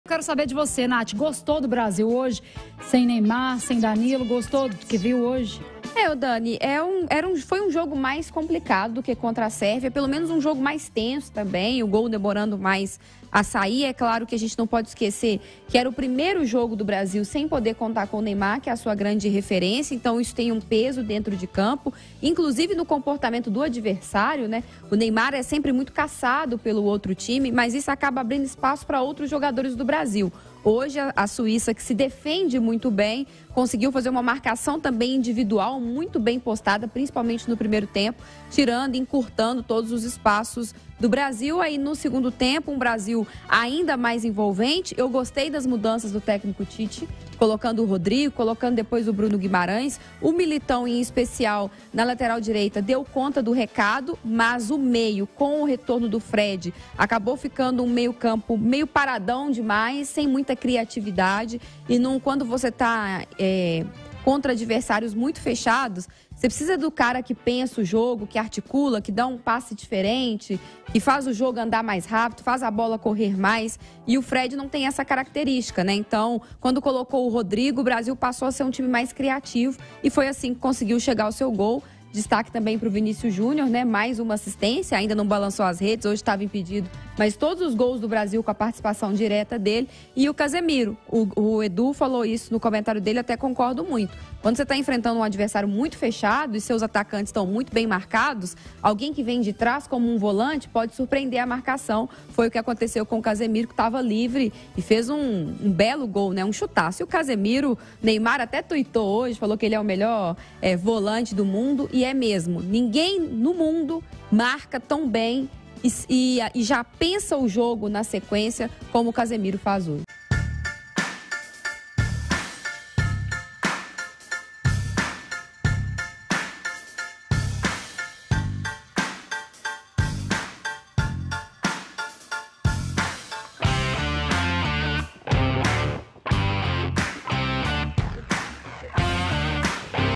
AUSÊNCIA DE NEYMAR COMPLICOU A PARTIDA?
O Brasil enfrentou a Suíça sem a presença de Neymar, machucado. A equipe teve dificuldades para criar com a falta do camisa 10, mas conseguiu se manter sólido na defesa e, com as substituições, voltou a ter domínio da partida.